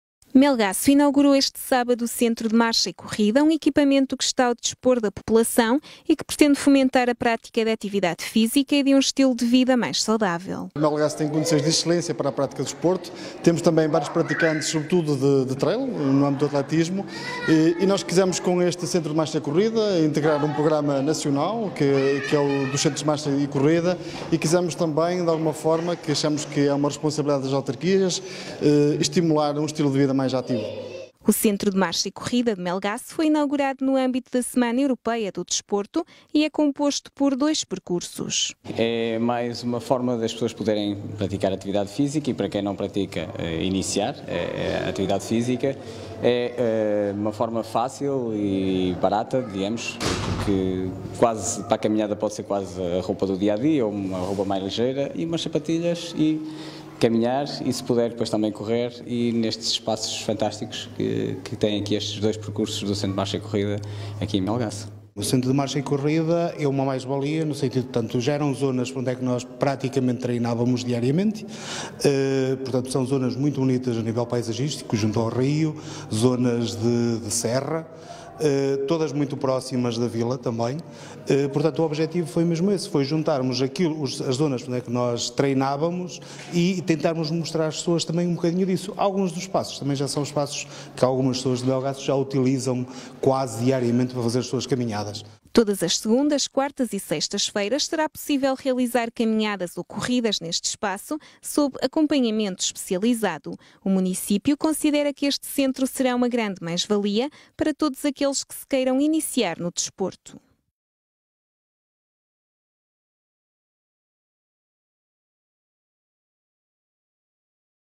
Inauguração do Centro de Marcha e Corrida de Melgaço
Inauguramos, no passado sábado, o Centro de Marcha e Corrida de Melgaço (CMCM), um espaço aberto a todos os que queiram iniciar ou manter hábitos de atividade desportiva 💚🚶‍♀️🚶‍♂️
DOIS PERCURSOS:
- Percurso Urbano, cujo piso é de asfalto/betuminoso/calceta, numa distância de 5.130 m e de dificuldade média/baixa.
- Percurso Natural, em asfalto/betuminoso e saibro/madeira, com uma distância de 5.050 m e dificuldade média/baixa.
Podem realizar caminhadas ou corridas nestes percursos às segundas, quartas e sextas, das 18h00 às 20h30. Terão sempre apoio e acompanhamento especializado por técnicos de educação física e desporto da Melsport – Melgaço, Desporto e Lazer, E.M. e da Melgaço Alvarinho Trail Team, de forma a orientar o atleta nos melhores e mais adequados exercícios para a sua condição física :)